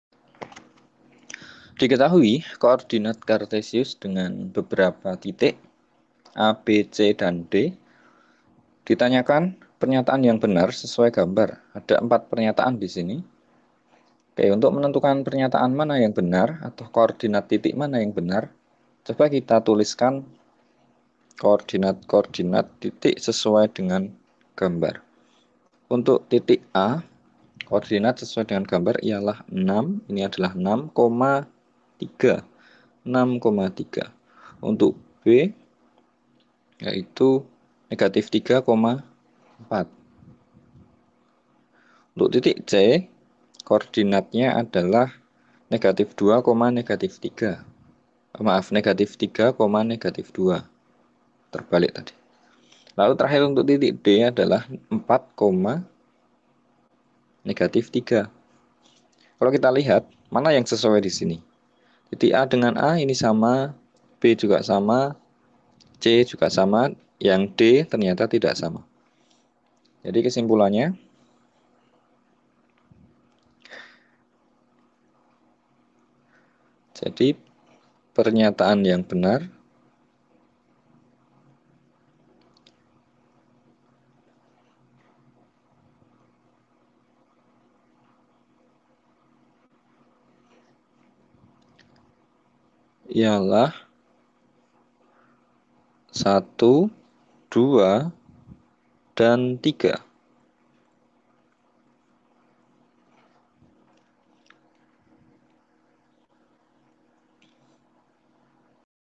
Cermati beberapa pernyataan berikut Titik A mempunyai koordinat 6,3|MTK SMP Kelas8|Uji Kompetensi1|1
✅ Dalam video ini kita akan membahas:
Cermati beberapa pernyataan berikut Titik A mempunyai koordinat 6,3

Topik: Koordinat Kartesius
Kelas: 8
Mata Pelajaran: Matematika
Bab: 2
Buku: PR Matematika Untuk SMP/MTS Kelas VIII Semester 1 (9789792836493)